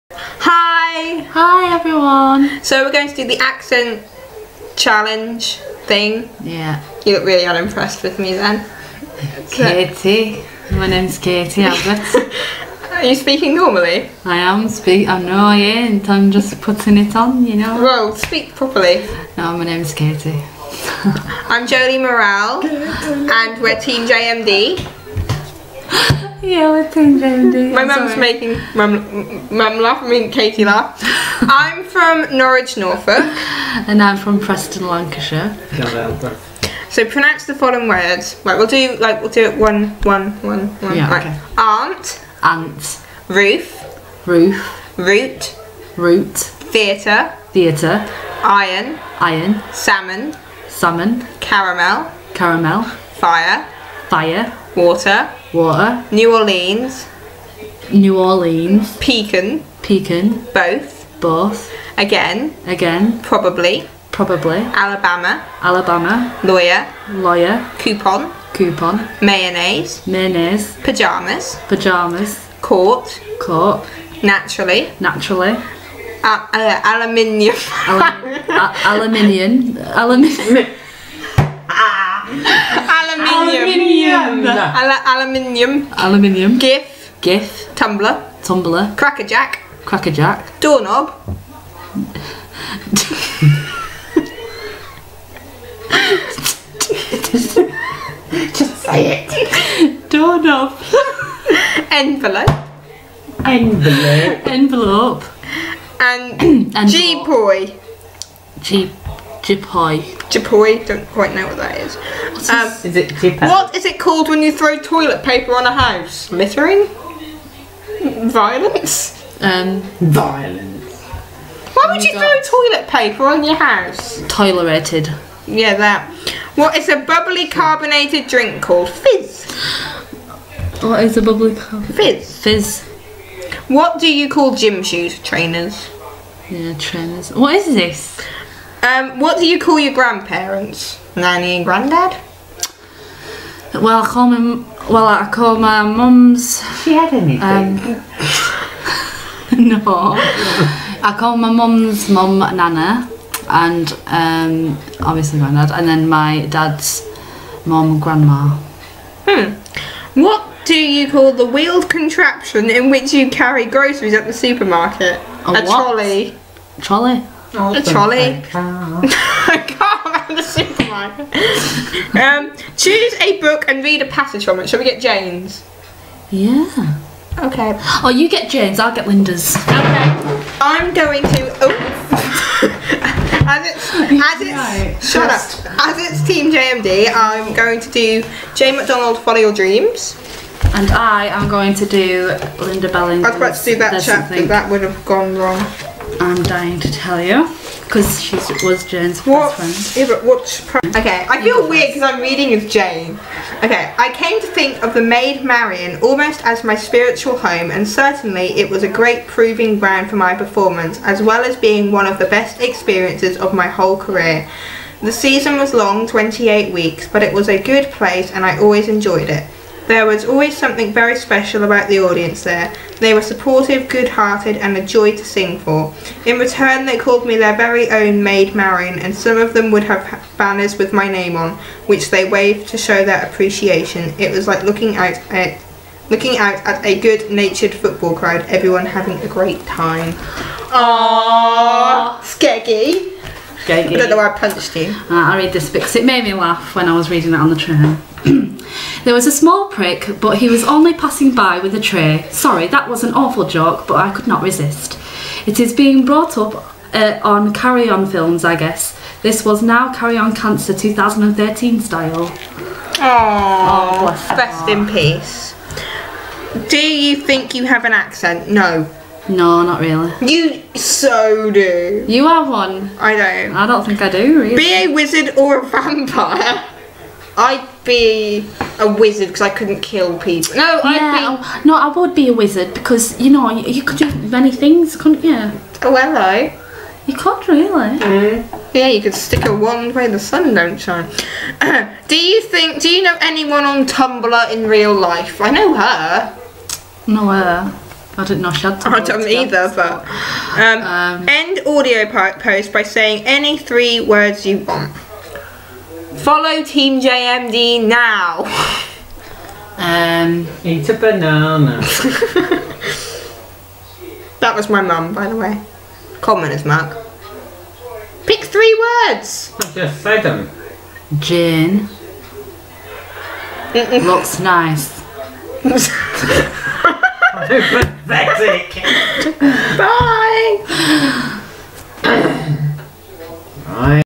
The Accent Tag Challenge - TeamJMD - Lancashire Vs Norfolk
Seeing as we both have completely different accents we thought we'd do the accent tag challenge.